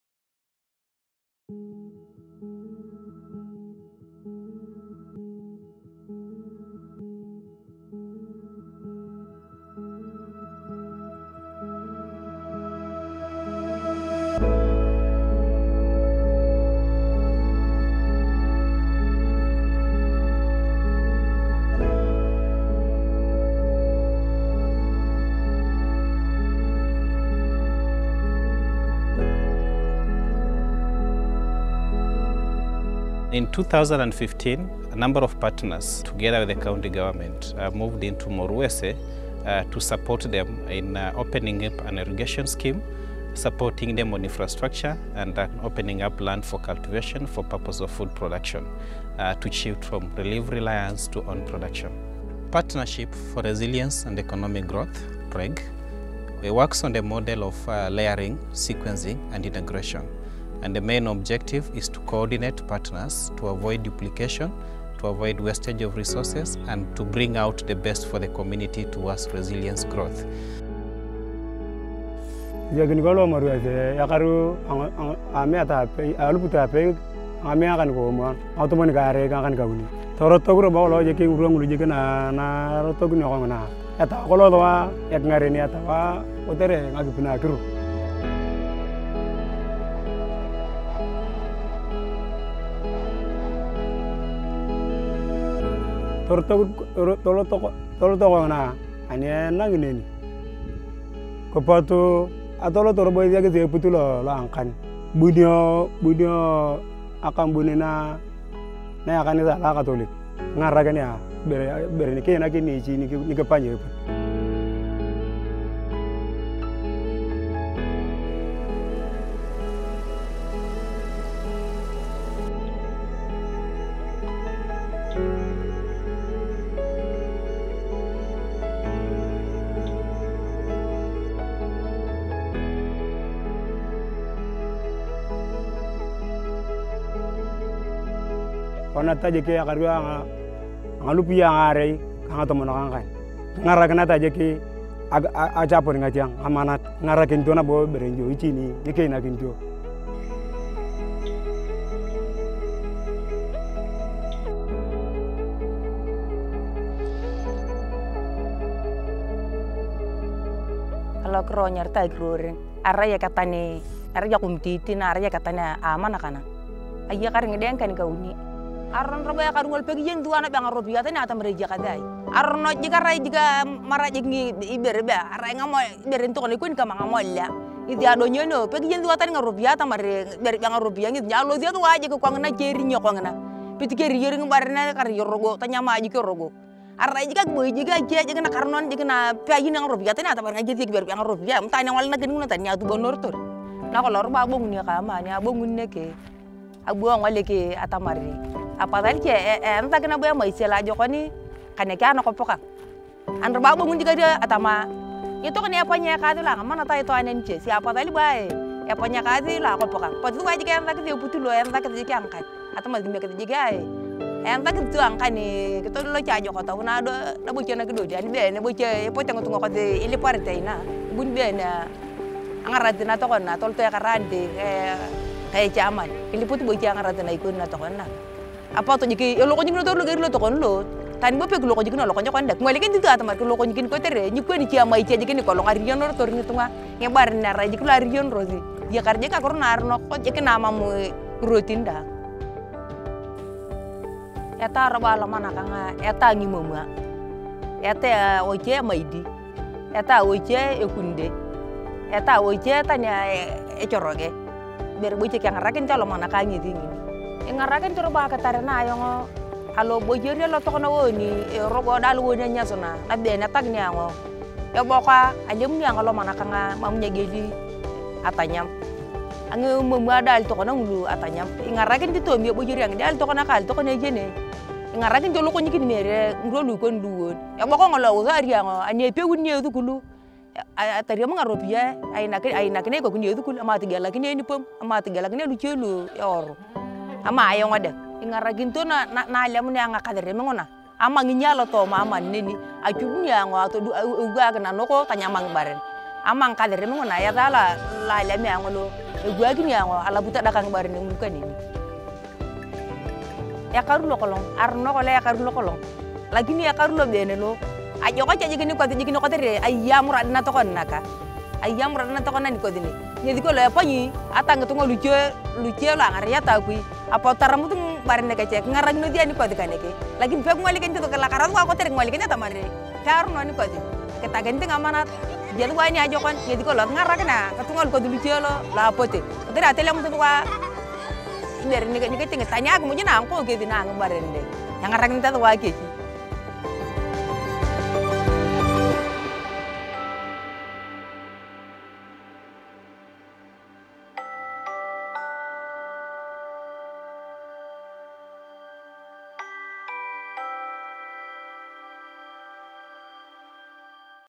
Turning the desert green
Spanning nearly 77,000 km², Northern Kenya’s arid Turkana County is the second largest and poorest county in Kenya. The county is home to nearly 1 million people, the majority of whom are pastoralists, relying heavily on livestock.

The Moruese village is located in Turkwel ward of Loima Sub-County of Turkana County. Located along the Turkwel River, the Moruese Irrigation Scheme was initiated in 1978 as a rain-fed farming project by 340 community members who wanted to introduce crop farming as a new livelihood activity in a community that reared livestock as its main livelihood activity. However, cycles of inadequate rainfall in the region over many years led to poor crop yields and the community eventually neglected the land.

In May 2015, USAID’s Partnership for Resilience and Economic Growth (PREG) moved in to revive the scheme after it was identified as a site that could be utilized to increase crop production and improve food security at the household and community level. The scheme is addressing the need for continuous water supply and improved agricultural and water management practices to enhance the livelihoods of community members who have been reliant on humanitarian assistance.